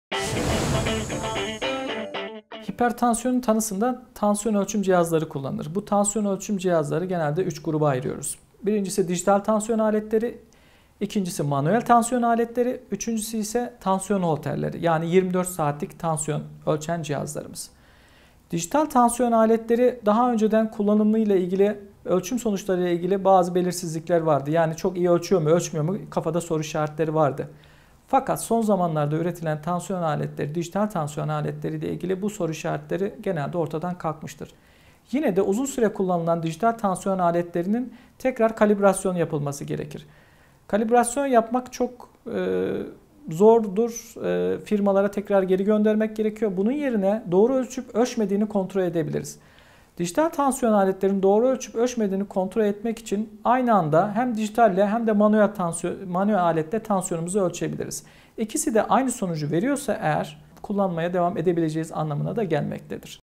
Hipertansiyon tanısında hangi testler kullanılır?
Hipertansiyon tanısında tansiyon ölçüm cihazları kullanılır. Bu tansiyon ölçüm cihazlarını genelde üç gruba ayırıyoruz. Birincisi dijital tansiyon aletleri, ikincisi manuel tansiyon aletleri, üçüncüsü ise tansiyon holterleri, yani 24 saatlik tansiyon ölçer cihazlarımız. Dijital tansiyon aletleri daha önceden kullanımıyla ilgili ölçüm sonuçlarıyla ilgili bazı belirsizlikler vardı. Ölçüyor mu ölçmüyor mu kafada soru işaretleri vardı. Fakat son zamanlarda üretilen dijital tansiyon aletleriyle ilgili bu soru işaretleri genelde ortadan kalkmıştır. Yine de uzun süre kullanılan dijital tansiyon aletlerinin tekrar kalibrasyonunun yapılması gerekir. Kalibrasyon yapmak çok zordur. Firmalara tekrar geri göndermek gerekiyor. Bunun yerine doğru ölçüp ölçmediğini kontrol edebiliriz. Dijital tansiyon aletlerinin doğru ölçüp ölçmediğini kontrol etmek için aynı anda hem dijitalle hem de manuel aletle tansiyonumuzu ölçebiliriz. İkisi de aynı sonucu veriyorsa eğer bunlar kullanmaya devam edebileceğimiz anlamına gelir.